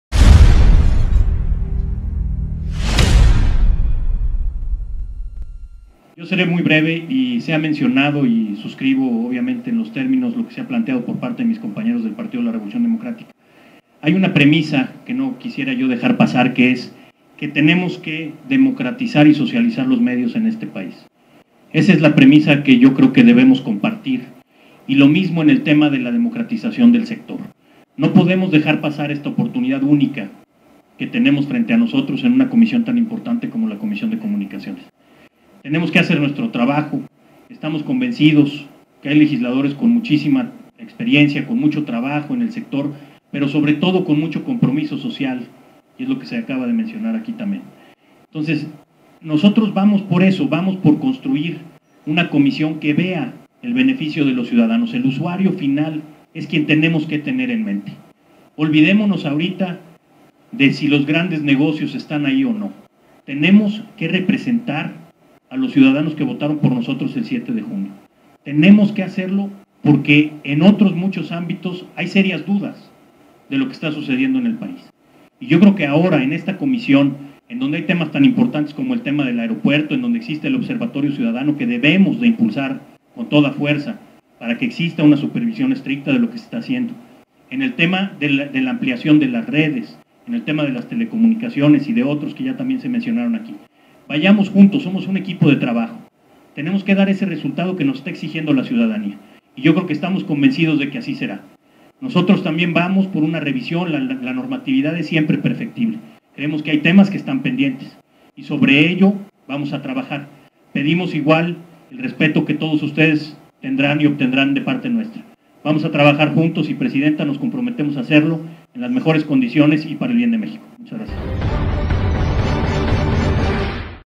Int Diputado Francisco Xavier Nava - Comisión de Comunicaciones
Int del Diputado Francisco Xavier Nava en la  Reunión de Instalación de la Comisión de Comunicaciones.